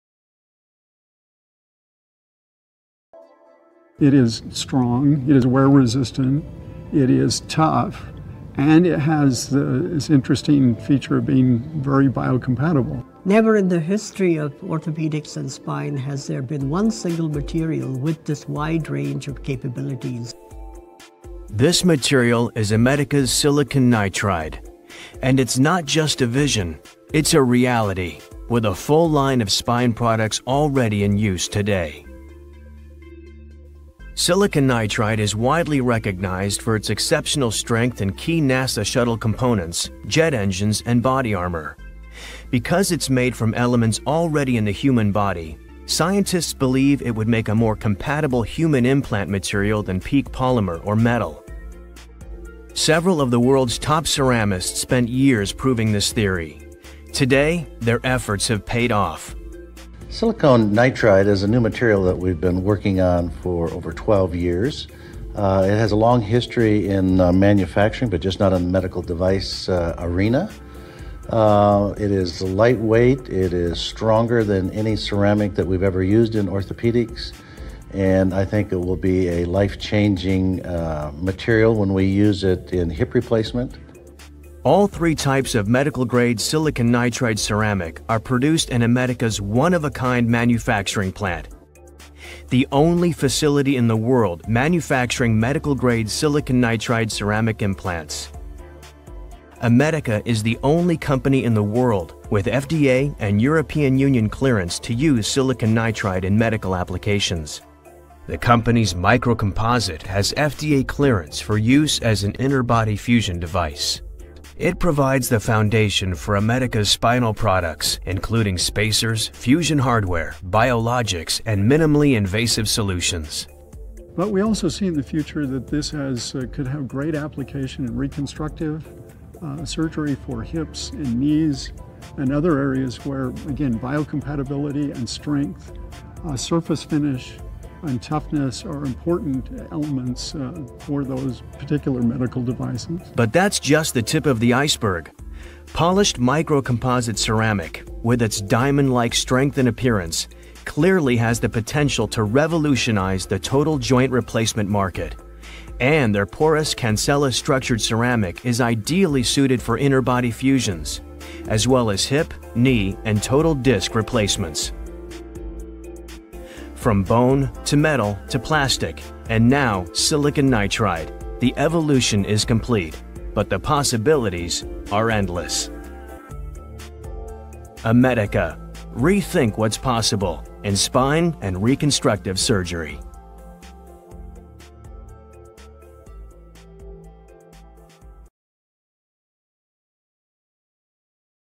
The Silicon Nitride Manufacturing
All three types of medical grade silicon nitride ceramic are produced in Amedica's one-of-a-kind manufacturing plant, the only facility in the world manufacturing medical grade silicon nitride ceramic implants. The manufacturing process of silicon nitride, a material used in orthopedic and spinal implants.